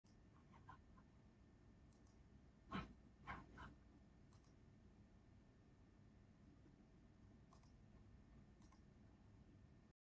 Classic Trip Animation
This is my first pencil 2d animation, hope you like it! :D